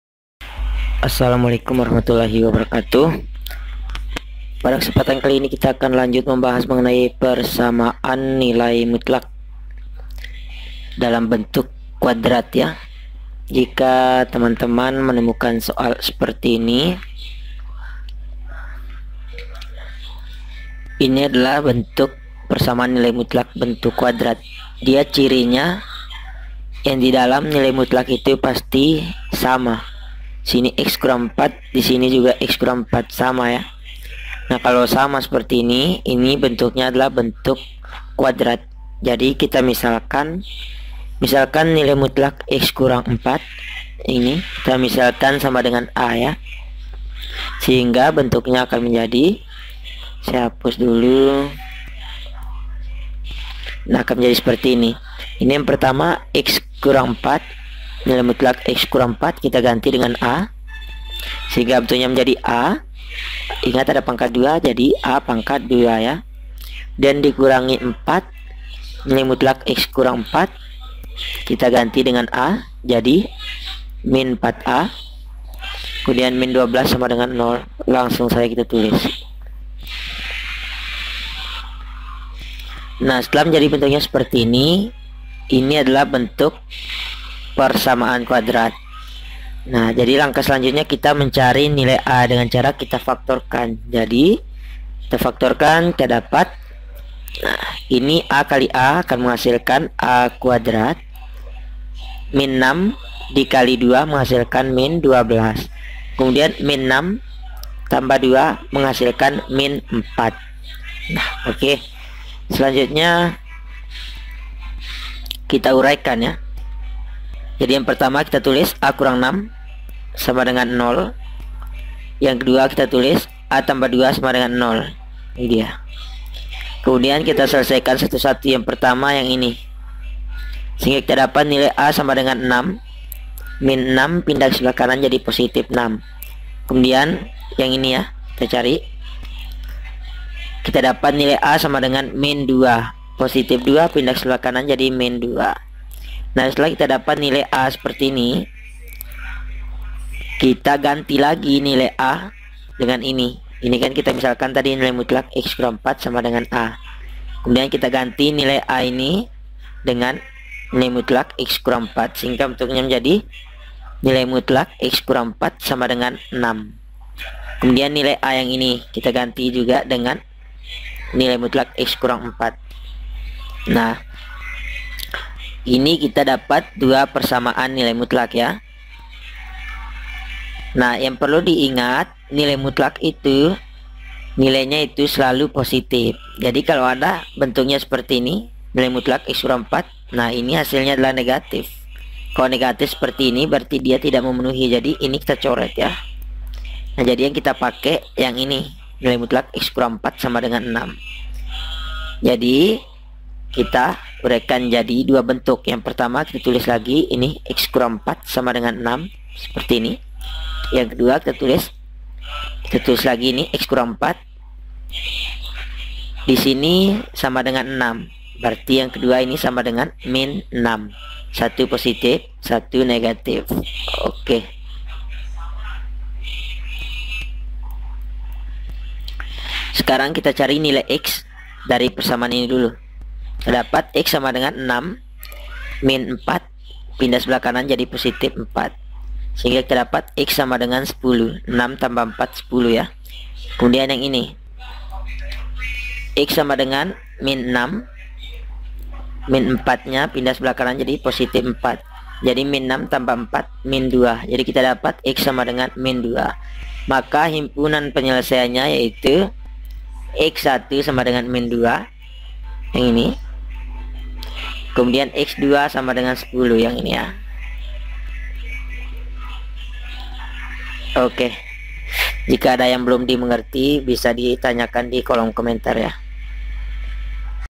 Cara Mudah Selesaikan Soal Nilai Mutlak Bentuk Kuadrat Part 2
Pada vidio ini Fan Mathematic akan menjelaskan cara mudah dalam menyelesaikan soal persamaan nilai mutlak bentuk kuadrat matematika wajib kelas x sma.